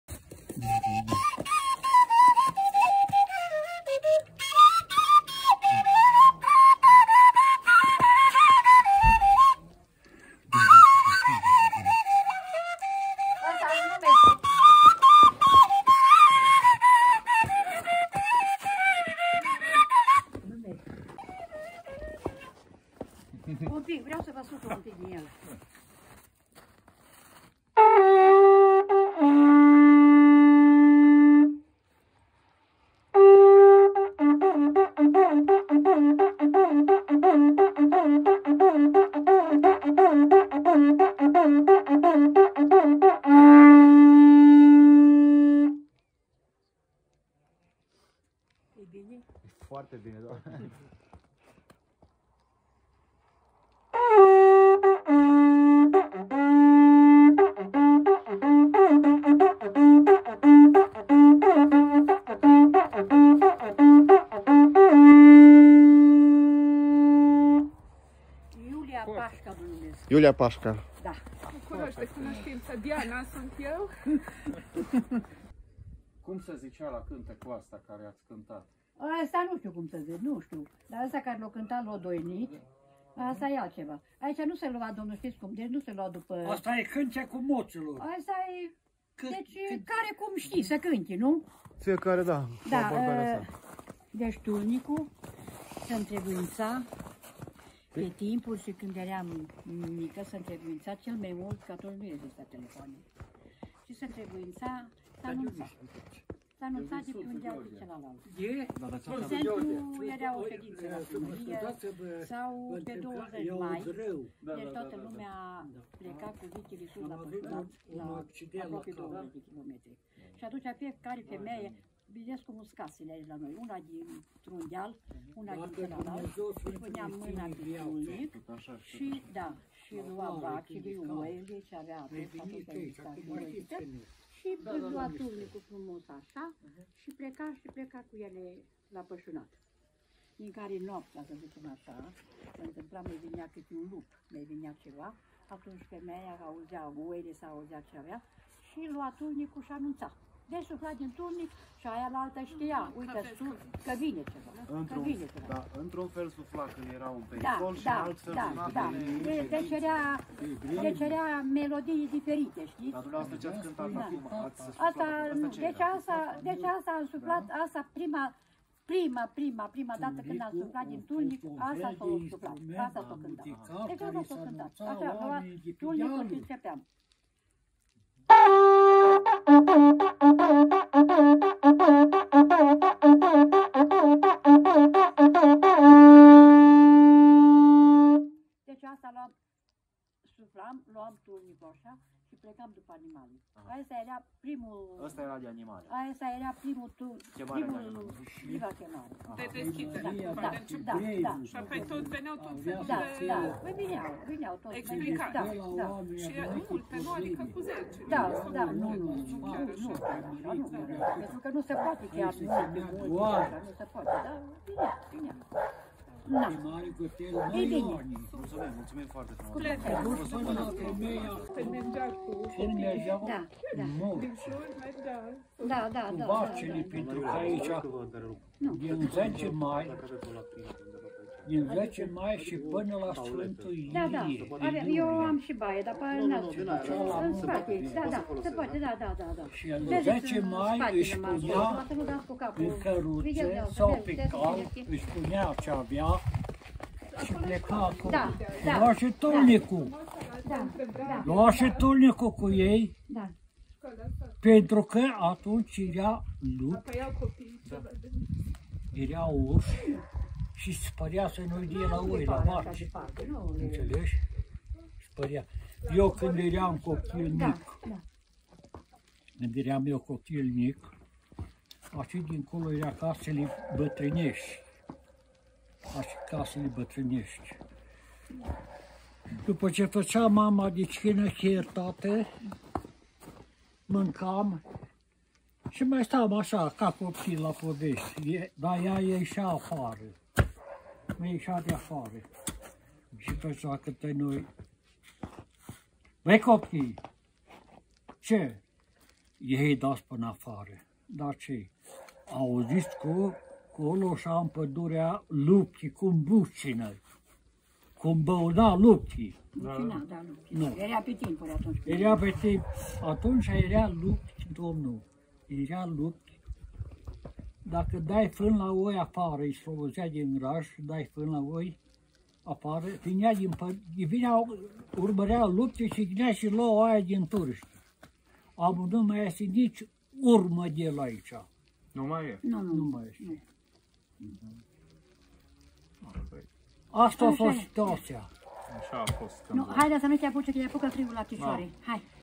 Fluier și tulnic în satul Ghețari la familia Pașca
În luna noiembrie a anului 2021 am vizitat familia Pașca din satul Ghețari, comuna Gârda de Sus din județul Alba, Transilvania, România. (Punct de acces spre Peștera Scărișoara)

Domnul Elisie Pașca, în vârstă de 86 de ani, a cântat la fluier, iar doamna Iulia Pașca la tulnic și ne-au povestit despre traiul de altădată.

Musafiri am fost: Birkoff Călin și Diana Grebleș, Ferenc Ségercz și Ivan Baraban.